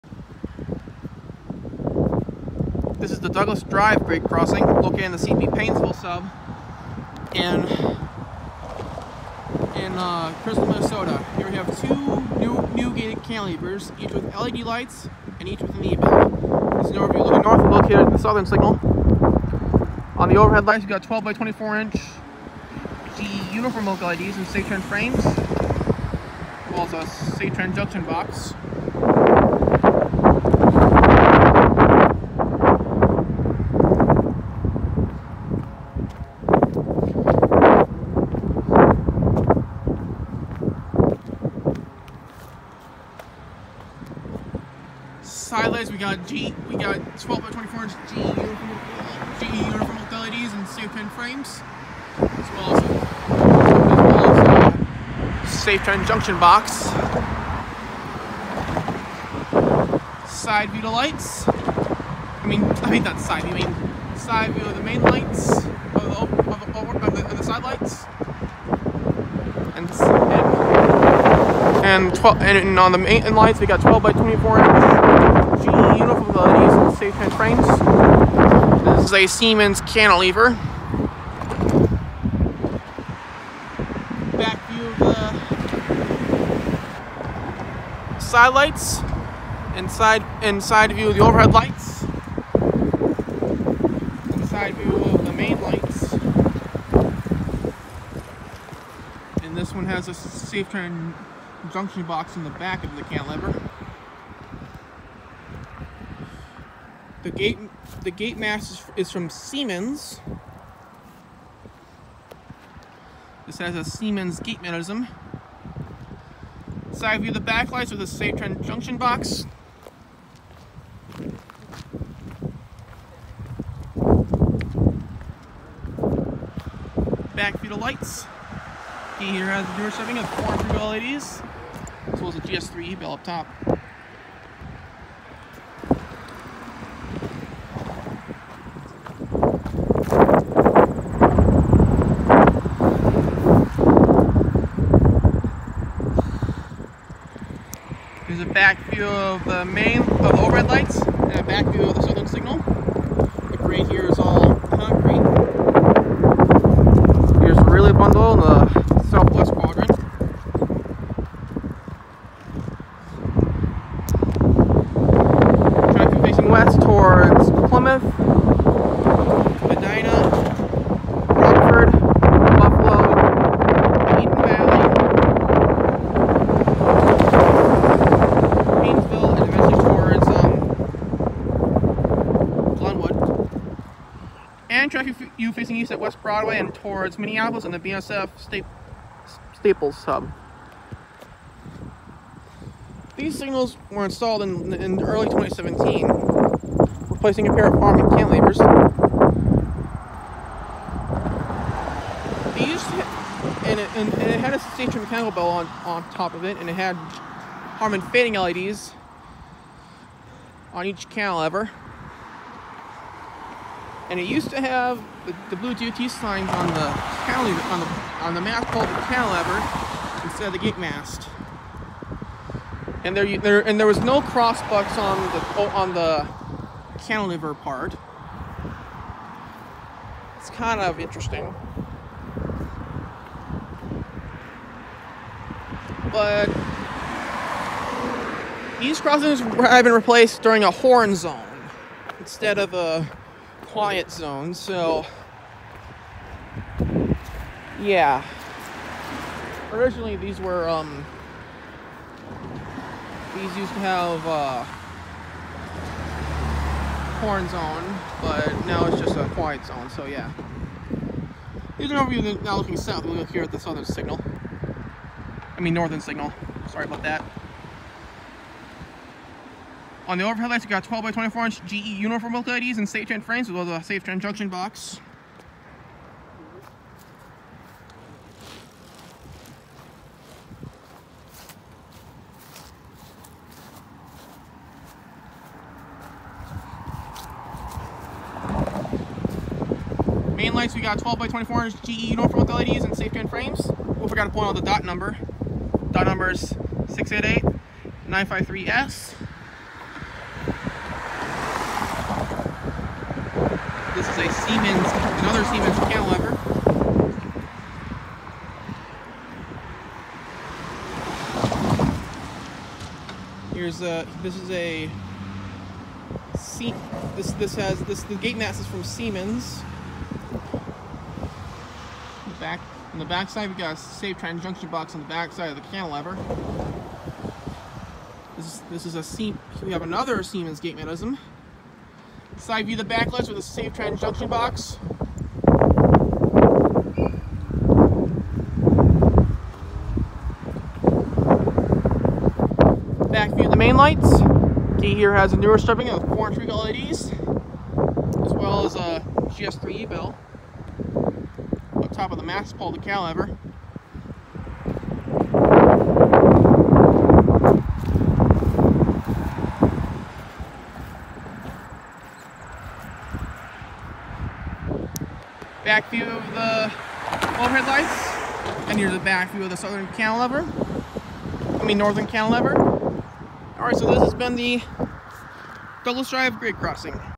Douglas Dr Grade Crossing Tour in Crystal MN
This Crossing is located on the CP Paynesville Sub. These Signals were installed in early of 2017 when this crossing had a horn zone. In late December of 2017 this crossing installed a quiet zone. That’s what I know about this crossing.

Southern Signal
Bell: GS Type 3 E Bell
Base: Siemens Cantilever, Siemens Gate Mast
Lights: 6 Pairs of GE Uniform look LEDs in Safetran Frames
Junction Boxes: Safetran
Gate Lights: 4 Inch RECO LEDs
Gate Mechanism: Siemens

Northern Signal
Lights: 4 Pairs of GE Uniform look LEDs in Safetran Frames
Junction Boxes: Safetran
Gate Mechanism: Siemens